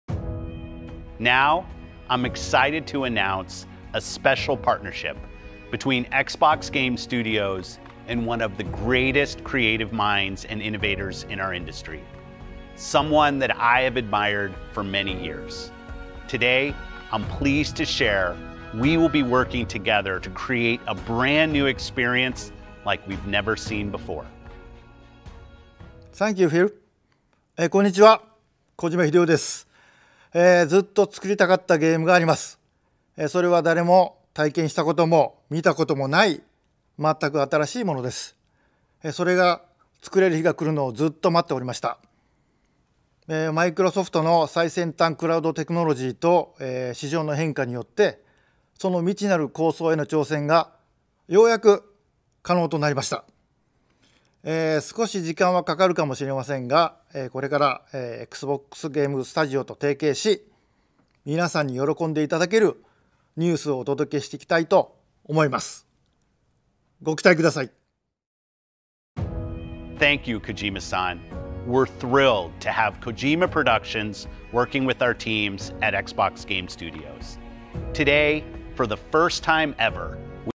Xbox Games Studios + Kojima Productions Announcement | Xbox & Bethesda Games Showcase 2022
Hideo Kojima announces he is teaming up with Xbox Game Studios to create a brand new game.